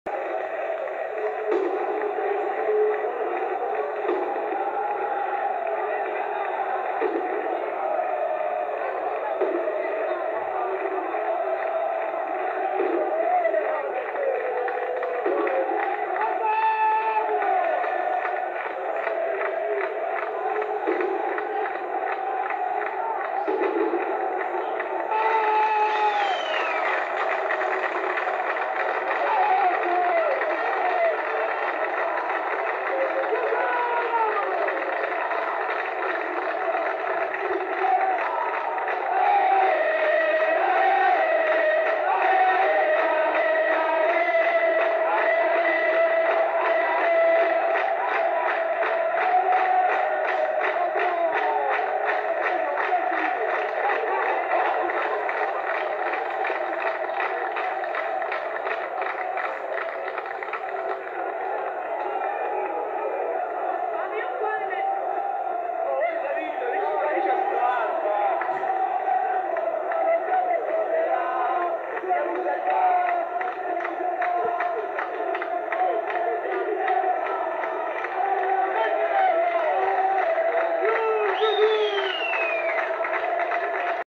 20 Maggio 2007 - Siracusa Calcio vs Savoia 1-0: l'atmosfera del De Simone di Siracusa nel prepartita
Autore del video Adrywolf